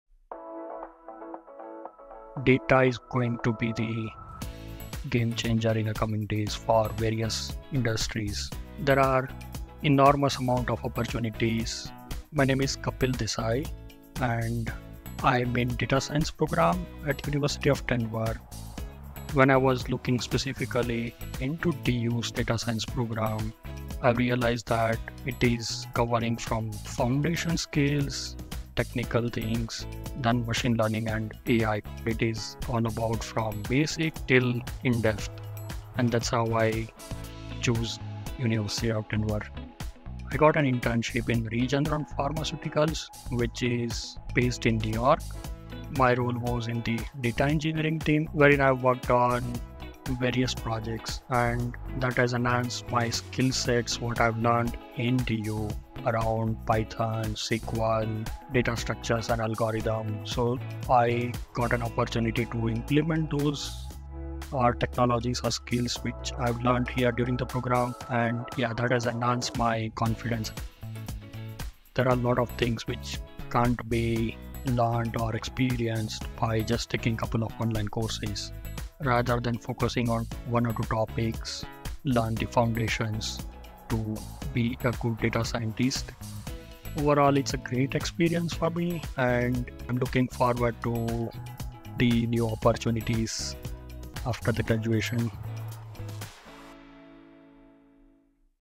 Ritchie School Alumni Spotlight: Kapil Desai's Journey to a Masters Degree in Data Science
In this alumni spotlight, we chat with Kapil Desai, a distinguished graduate of the Ritchie School of Engineering & Computer Science at the University of Denver. Kapil shares his journey from having limited knowledge about data science to becoming proficient in various machine learning and deep learning models. Discover how the Master's degree in Data Science empowered him to optimize processes and reduce human errors in practical industry applications. Learn how the University of Denver equips students with the skills and confidence needed to succeed in the ever-evolving field of data science and visit our masters in data science